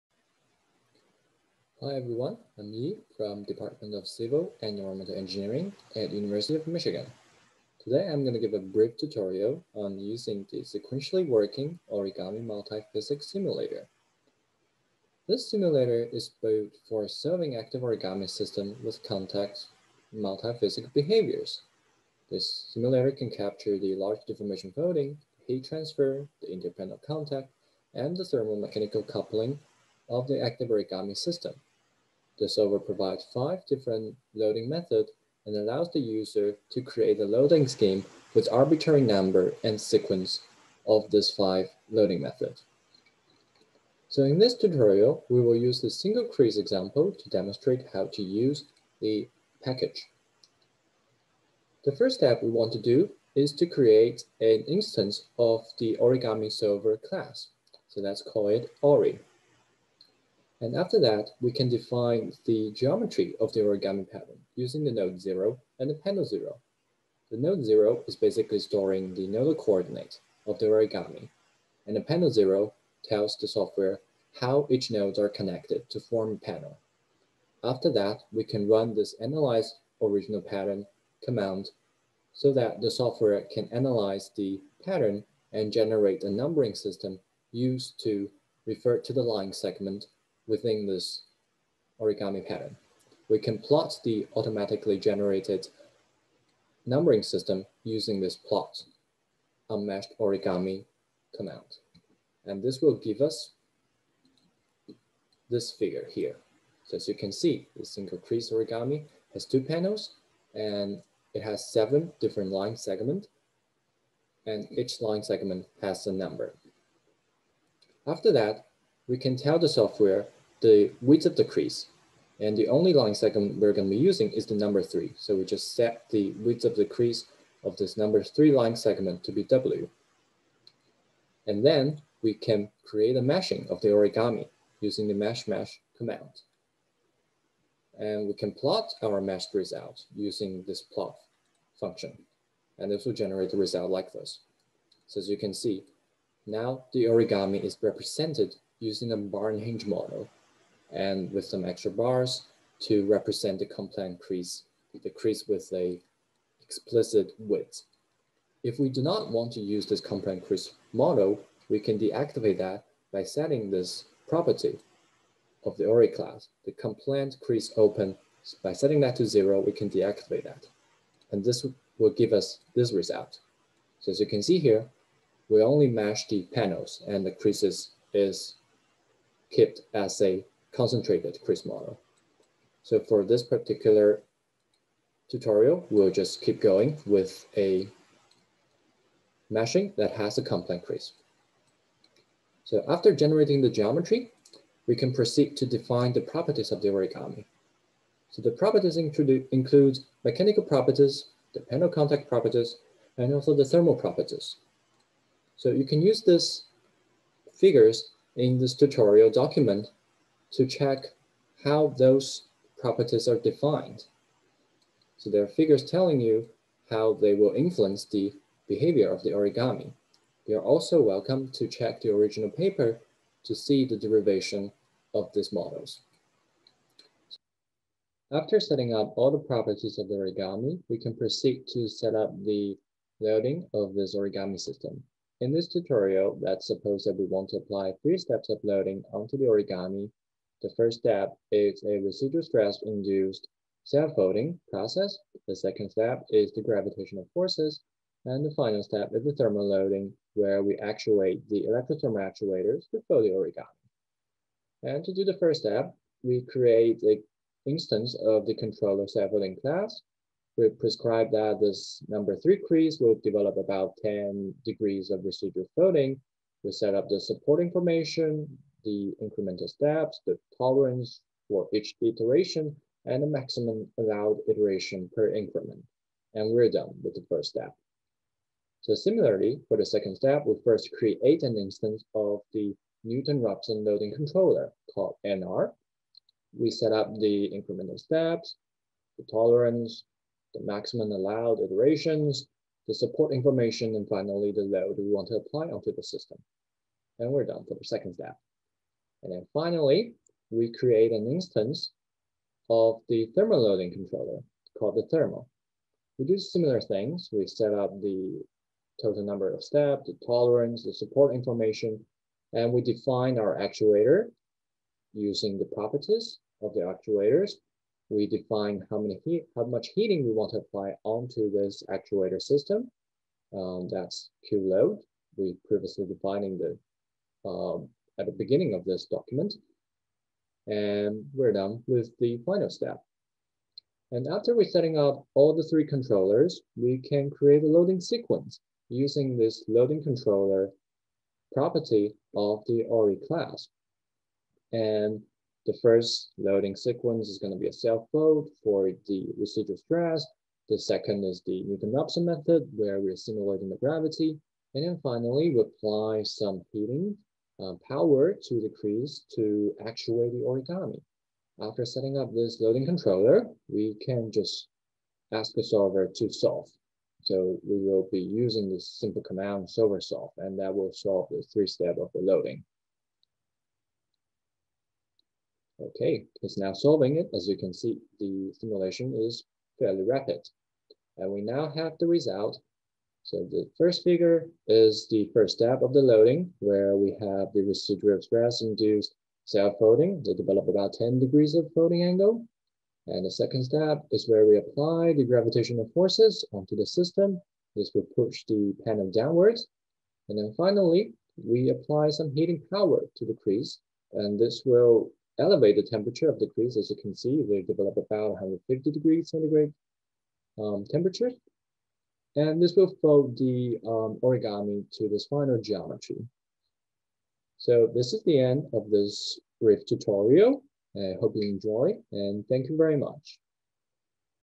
Tutorial of SWOMPS (Sequentially Working Origami Multi-Physics Simulator)
The SWOMPS simulator package can capture the complex multi-physics behavior of active origami systems.